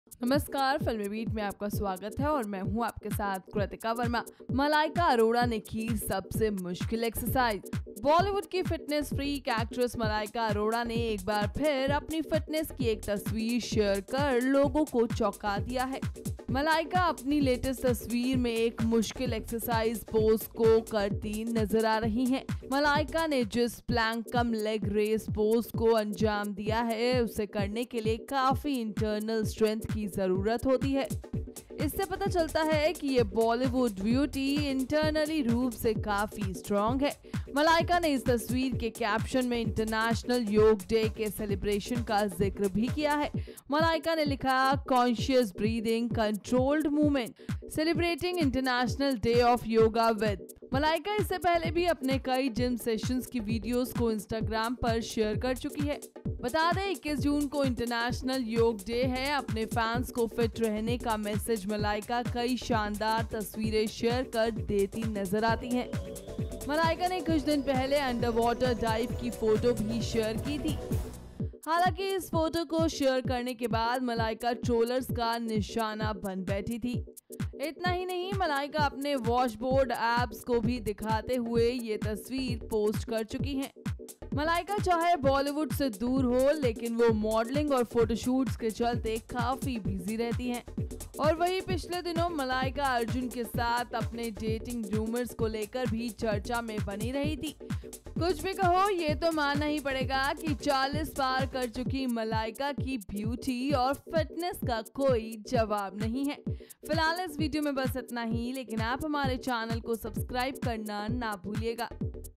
Malaika Arora working hard in gym, photo goes Viral । FilmiBeat
Malaika Arora is a fitness freak and one need not look beyond her workout videos to know her passion for the same.The 44-year-old actress has nailed some of the most difficult physical exercises in the past - from Pilates to barbell squats - and inspired us to hit the gym.This time, the fitness enthusiast left us dumfounded by giving us a glimpse of her acing yet another difficult workout.Malaika posted a picture of her on social media, in which, she is seen doing a combination of a plank and single leg raise.